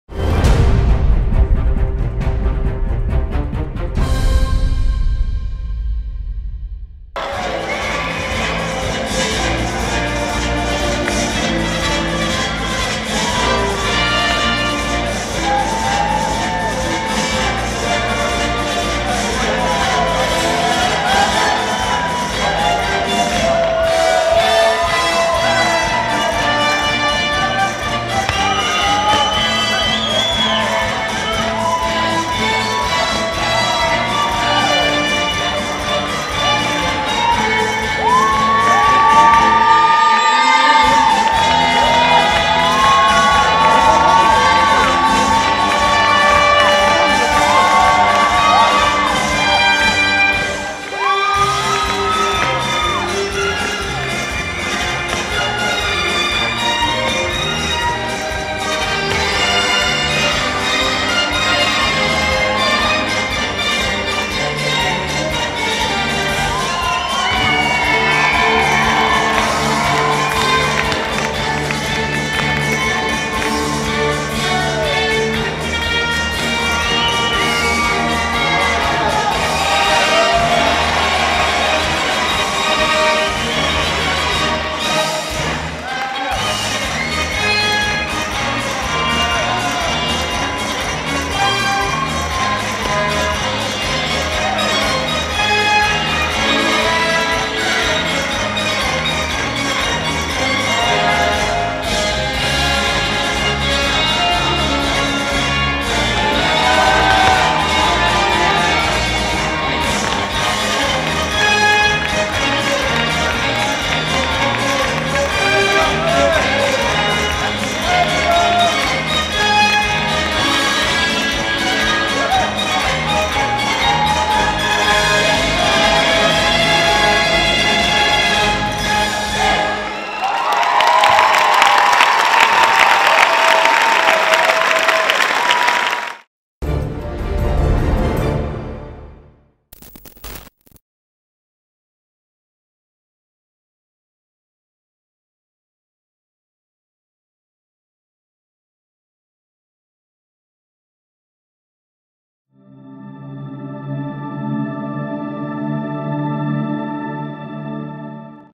Bangbang - Jing, CHN | 2015 World Latin R3 PD | DanceSport Total
Yan Bangbang - Zhu Jing, CHN, dance the Paso Doble in round 3 of the 2015 World Latin on 21 November in Vienna, AUT.